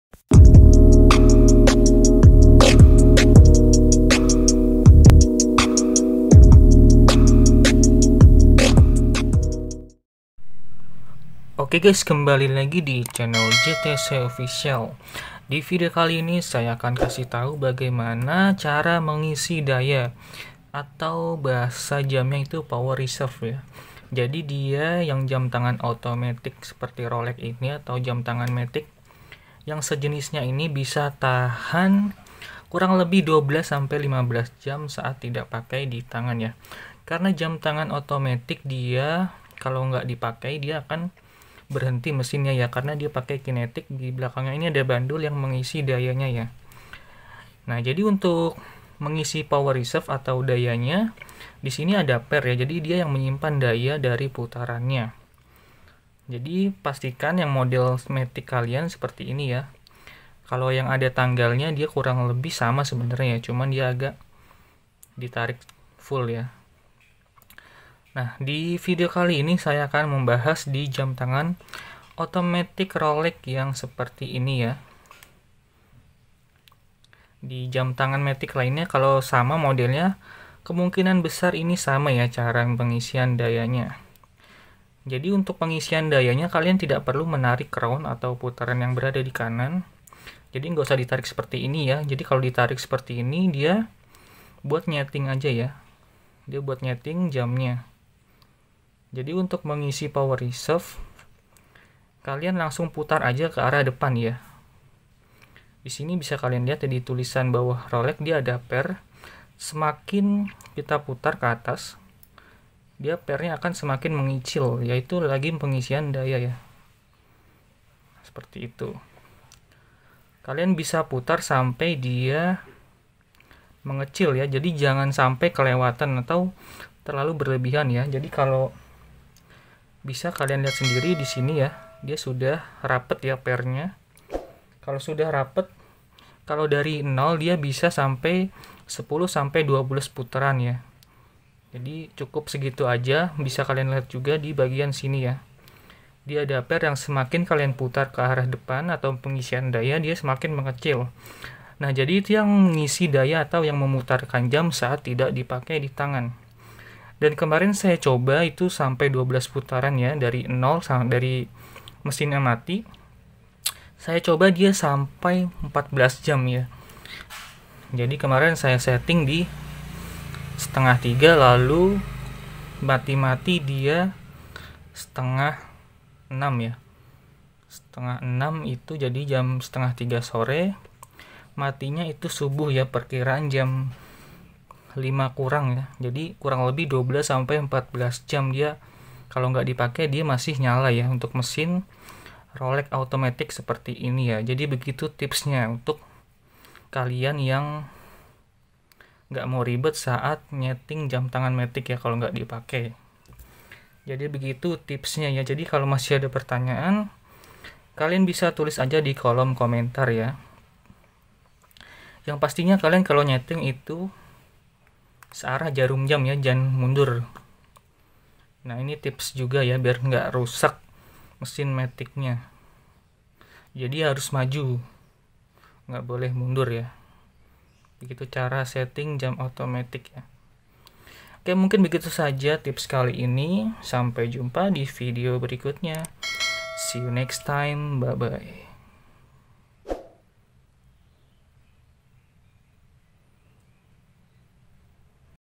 Cara Isi Daya Jam Tangan Automatic
Cara Isi Daya Jam Tangan Rolex Automatic

Power reserve merupakan fitur di mana sebuah movement jam tangan menyimpan energi yang didapatkan

Untuk daya tahan kalian bisa putar crown sebanyak 10-12x putaran sesuai dengan petunjuk yang ada di video.

Buat kalian yang ingin mendapatkan jam tangan ini bisa klik link ini